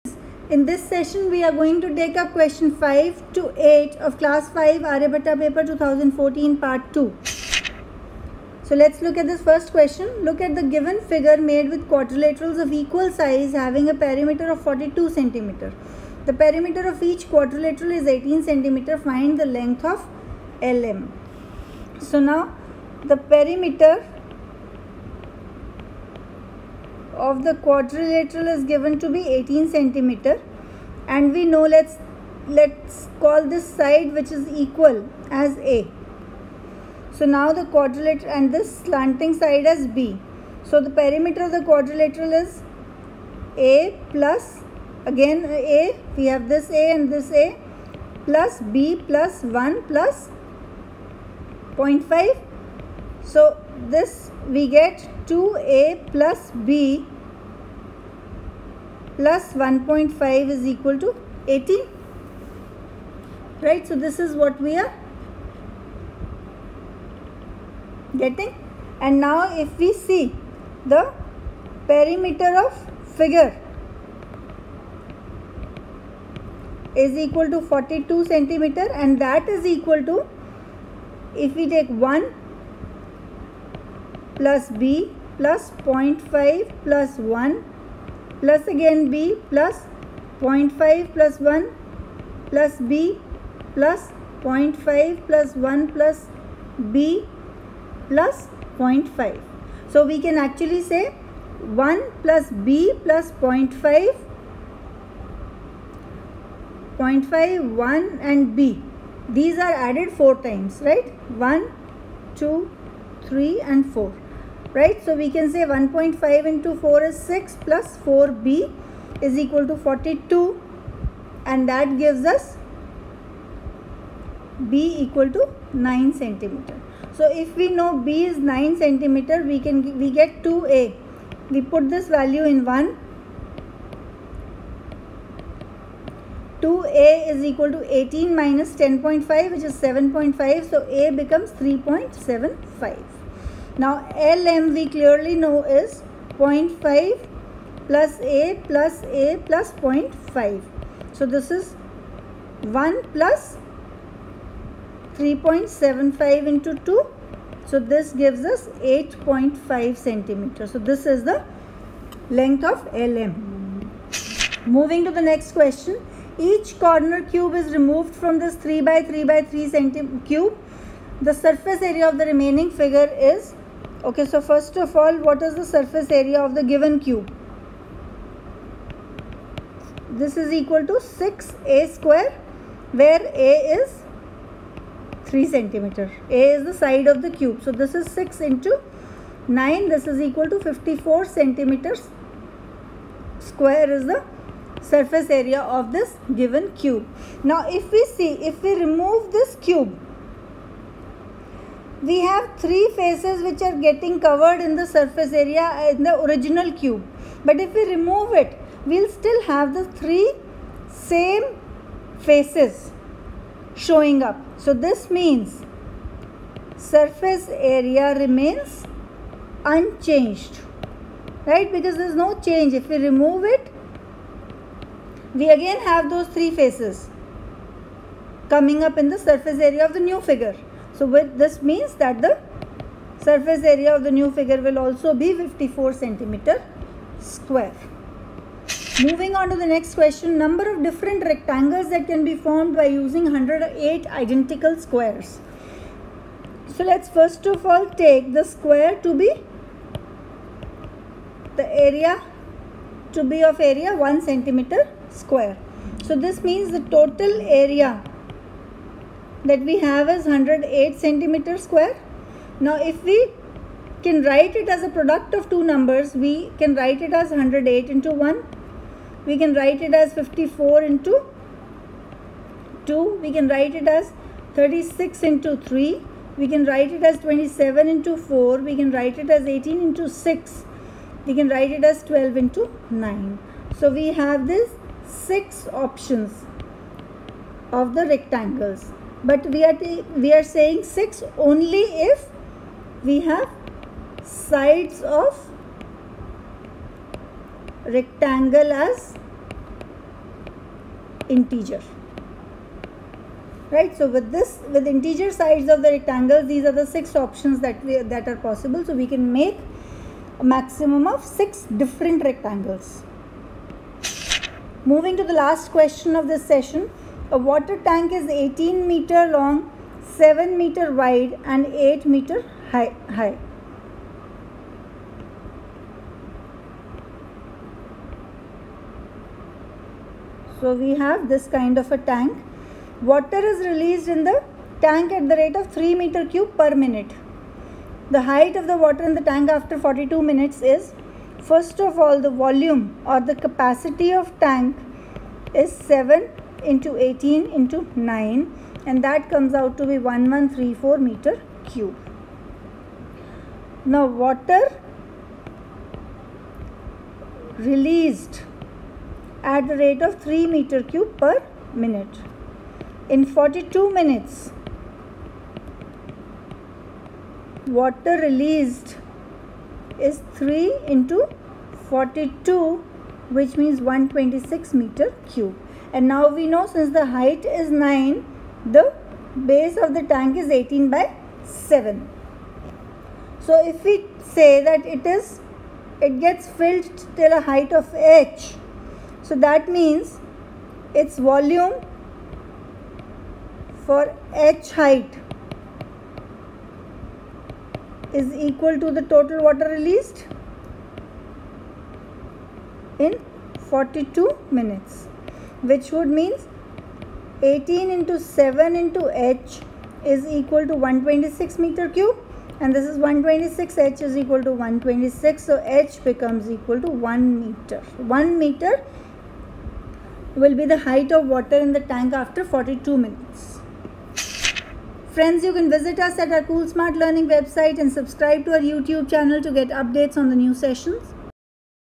Solution to Class 5 Aryabhatta Paper 2014 Part 2 Question 5 to 8
The video explains the solutions for Class 5 Aryabhatta Paper 2014 Part 2 Question 5 to 8.

You can also visit us on our and post your queries.